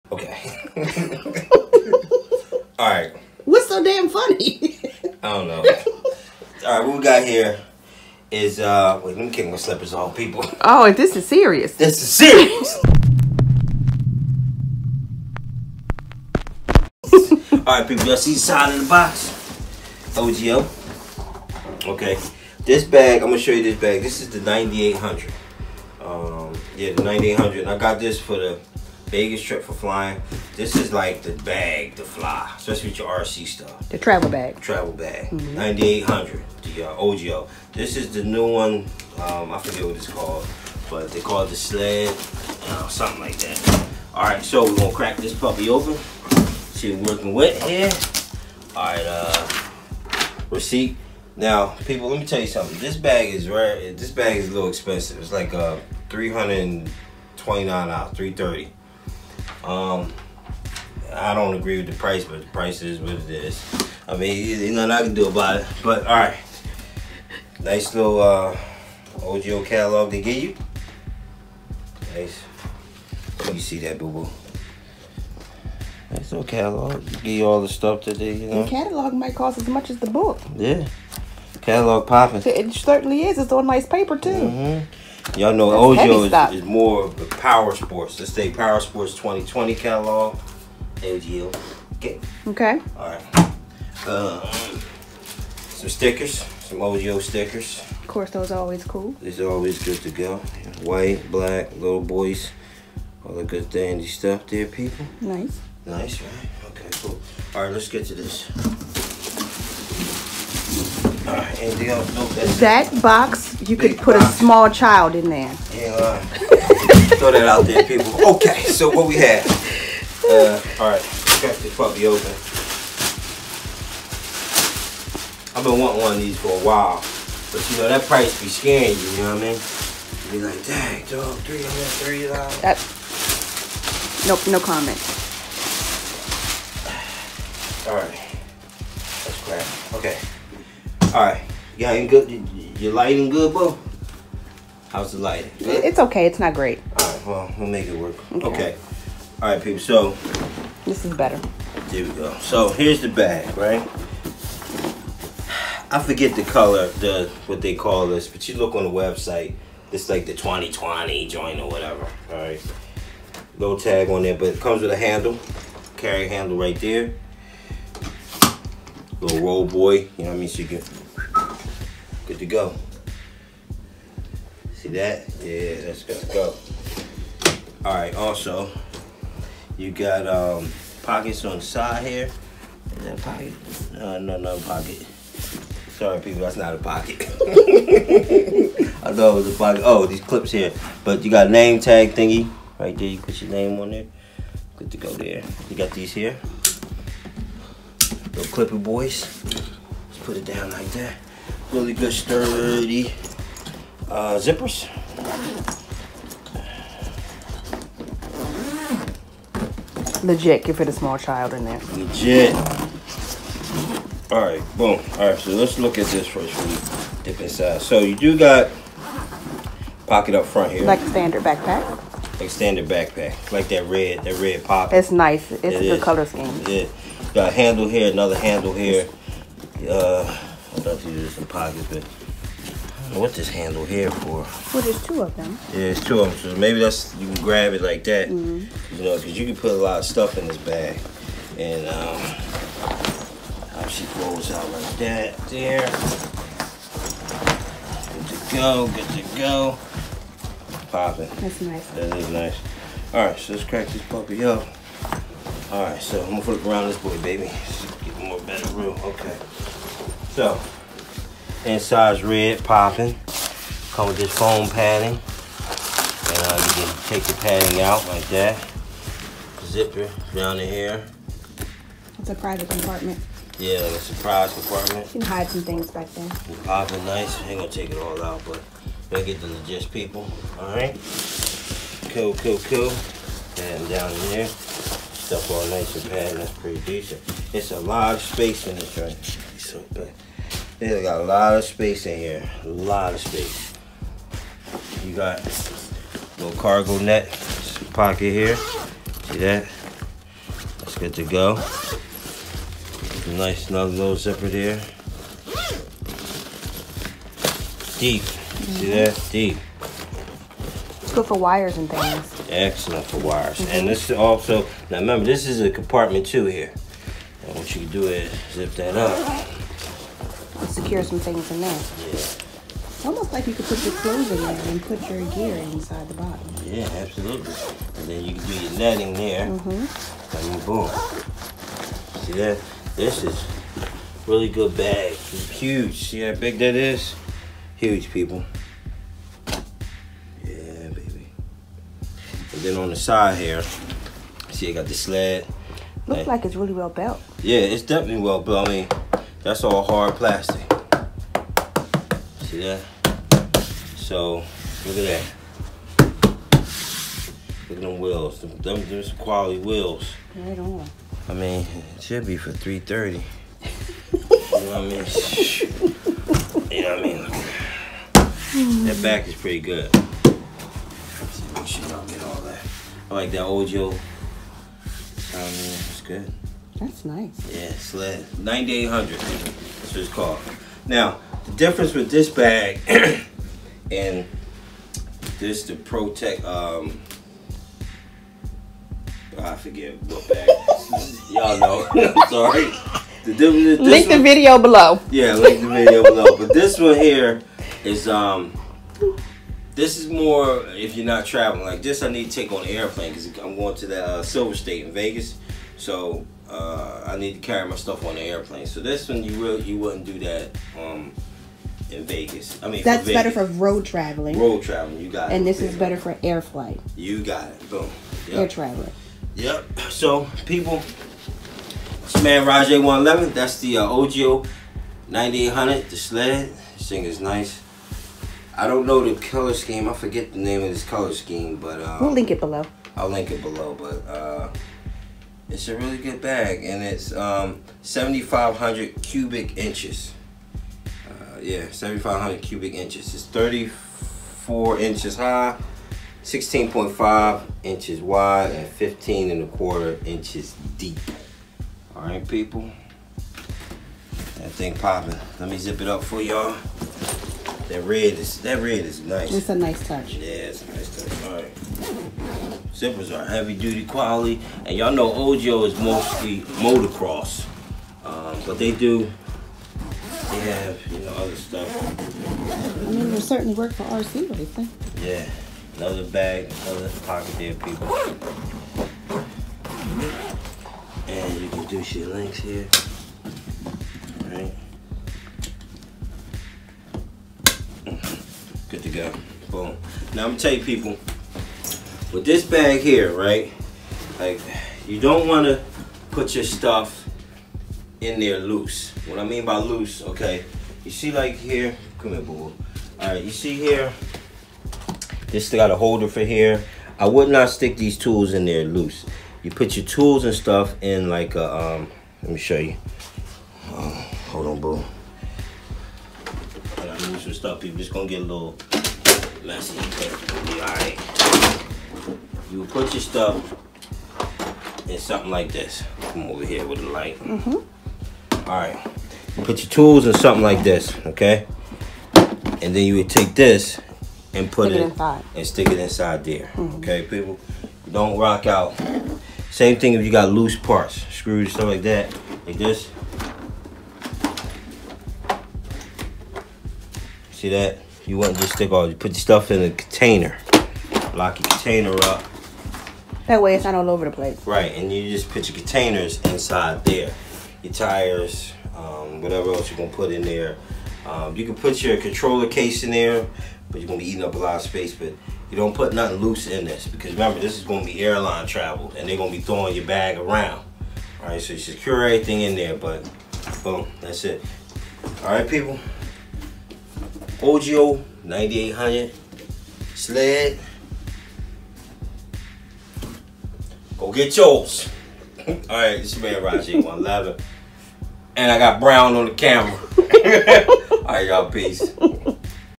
A look at the Ogio 9800 Travel Bag
Just picked up a Ogio sled 9800 special Ops 2020 travel bag, it's great for traveling or it can be used as a hauler bag. This will come in handy for the silver state race in Las Vegas, Nevada...